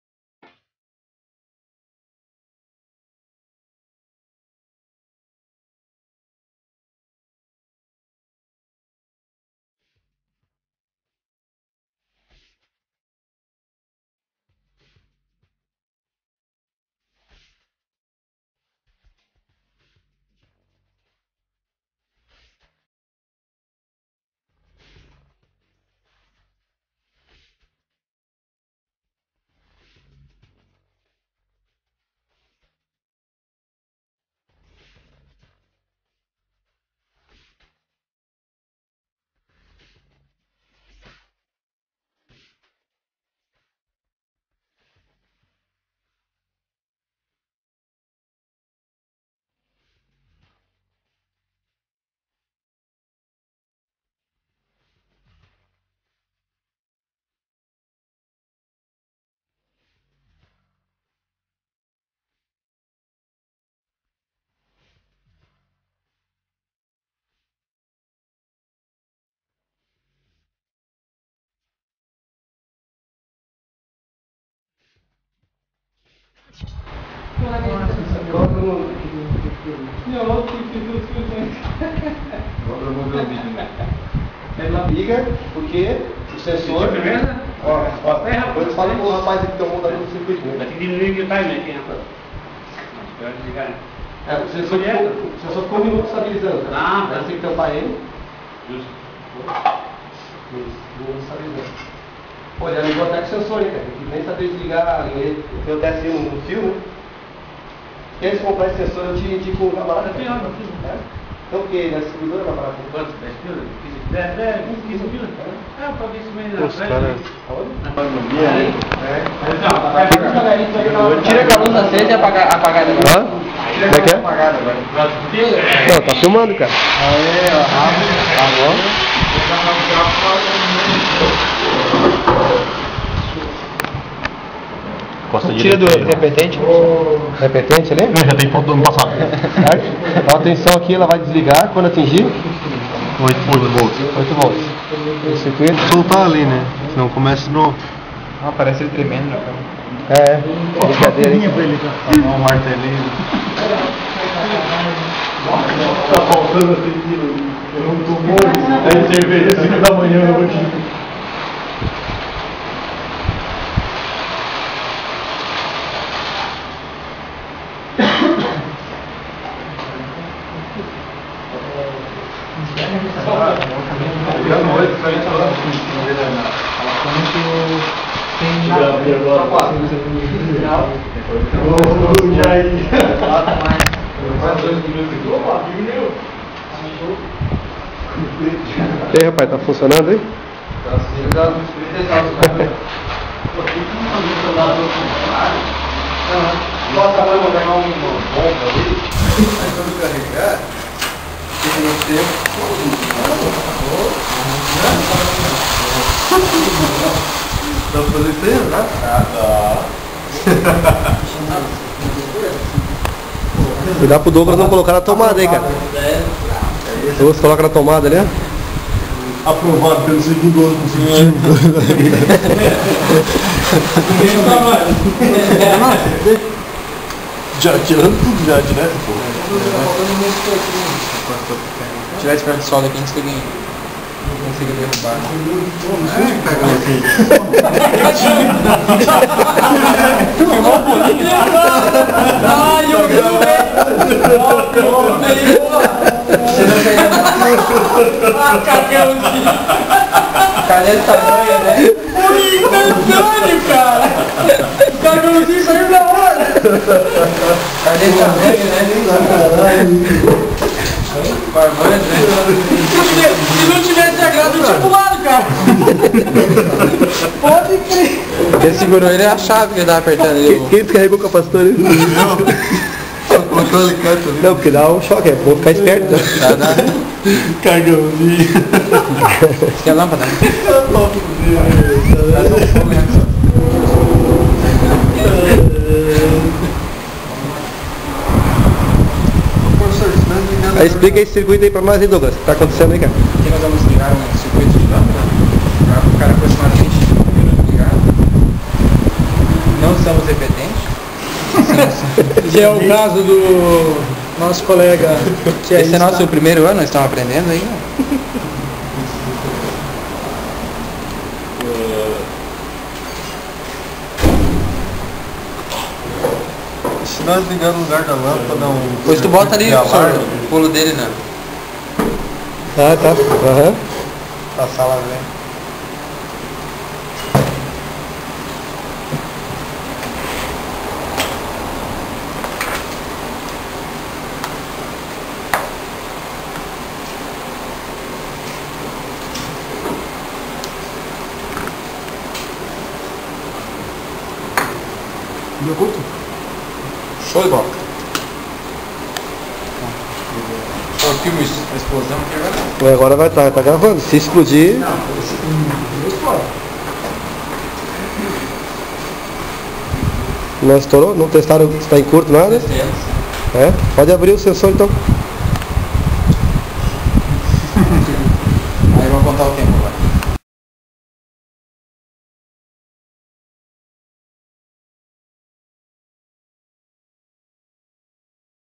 Turma 04 2010 Eletrônica Básica na Telredes
Alguns momentos de aula da turma do curso de eletrônica básica na telredes.